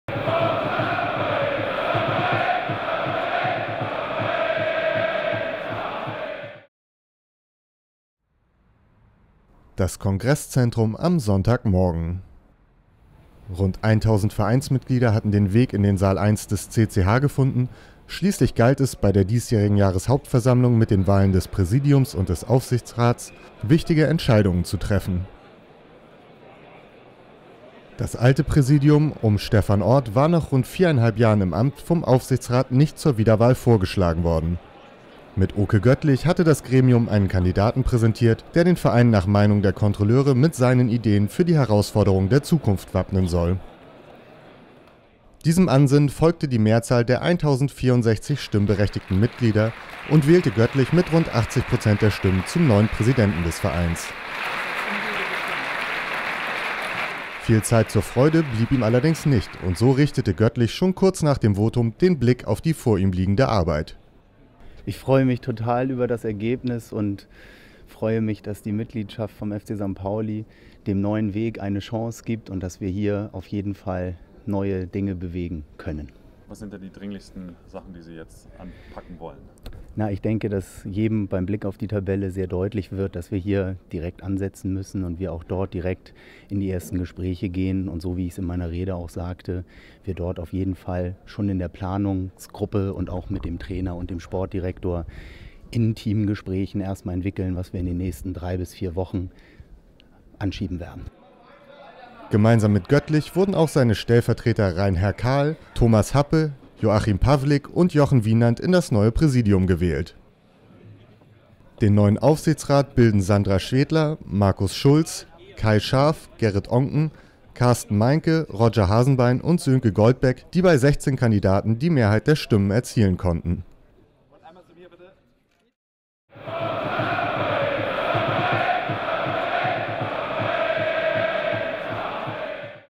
Das Wichtigste von der JHV 2014 I fcstpauli.tv
Natürlich waren wir auch bei der diesjährigen Jahreshauptversammlung mit der Kamera unterwegs. Den neuen Präsident Oke Göttlich im Gespräch mit der Presse findet Ihr bei fcstpauli.tv!